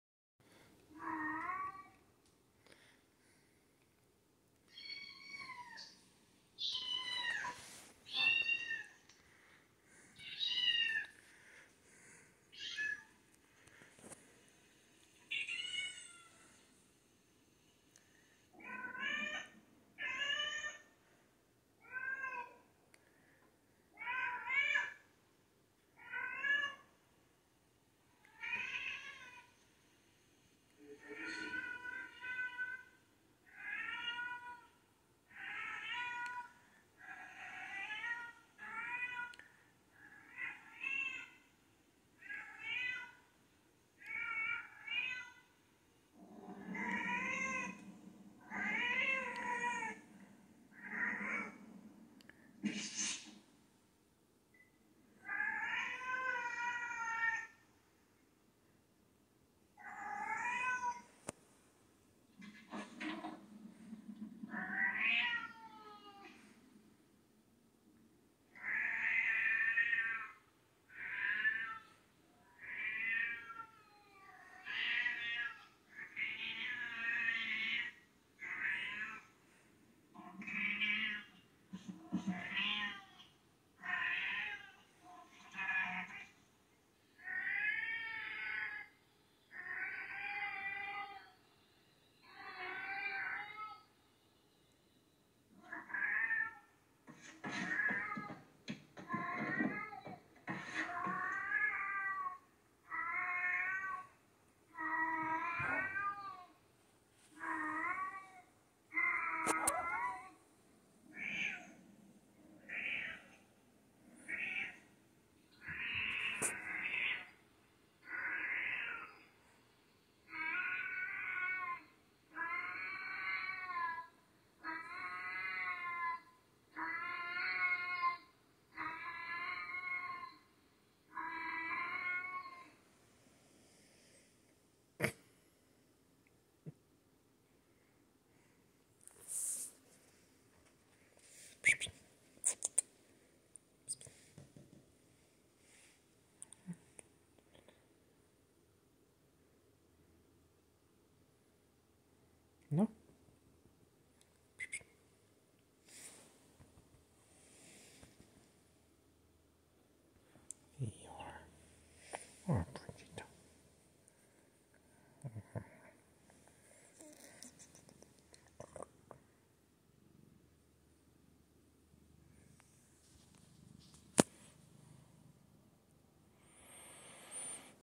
Cat watches screaming cat on TV
Dingus heard @shortyandkodi and had to see for herself